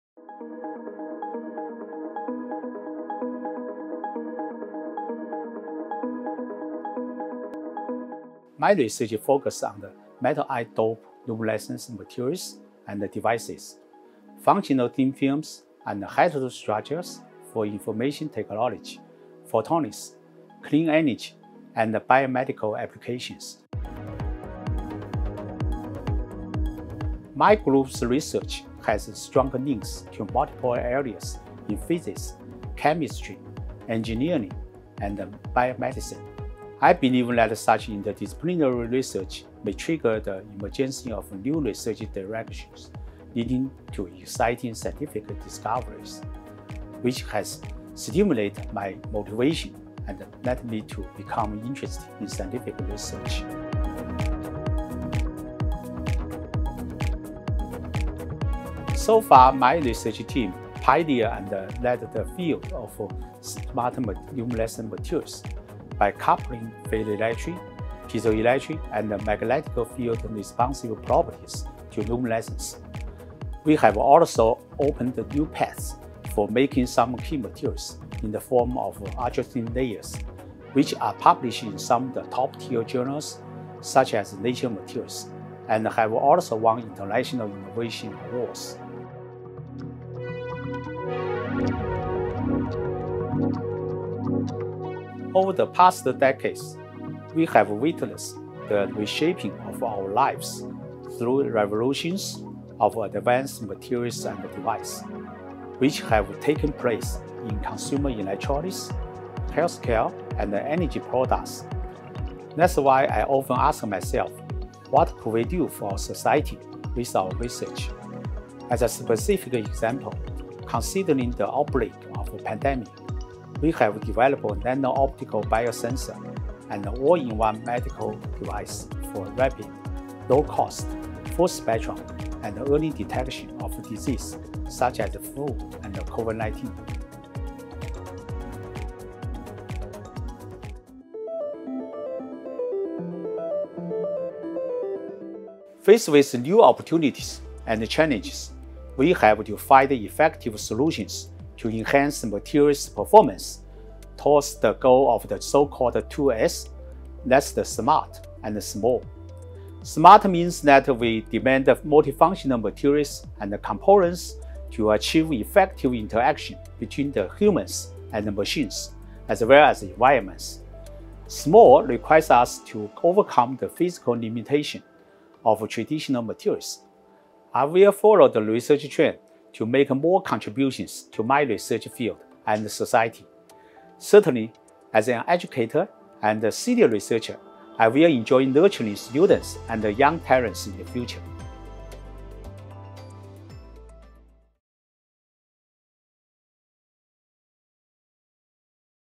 Meet PolyU Distinguished Scholar: Professor HAO Jianhua
Join Professor Hao Jianhua, Director of the Research Centre for Nanoscience and Nanotechnology and Chair Professor of Materials Physics and Devices of PolyU, as he explores the world of metal-ion doped luminescent materials and devices. In this video, Professor Hao shares his research in functional thin-films and heterostructures, spanning information technology, photonics, cleanenergy and biomedical applications.

Discover the profound impact of advanced materials and devices on consumer electronics, healthcare and energy products, reshaping our lives over the past few decades. Professor Hao’s research is driven by the question, “What can we do for society?” As a response to the pandemic, his team has developed nano optical biosensors and all-in-one medical devices for the rapid, low-cost, full-spectrum and early detection of diseases such as flu and COVID-19.

Chapters:
0:00 Opening
0:06 Research Domain and Current Focus
0:54 Key Research Achievements
1:31 Research Impact
2:22 Long-term Goals and Future Research Plans